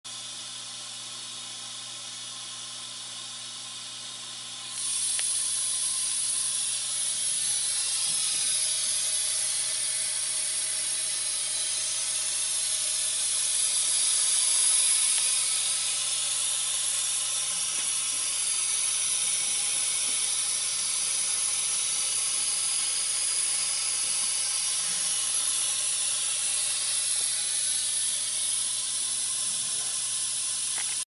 C8846 Used Videojet Excel 100 Ink Jet Coder SIGMA Equipment
This is a functional video of C8846 Used Videojet Excel 100 Ink Jet Coder.To view full description visit us at sigmaequipment.com for more available equipment.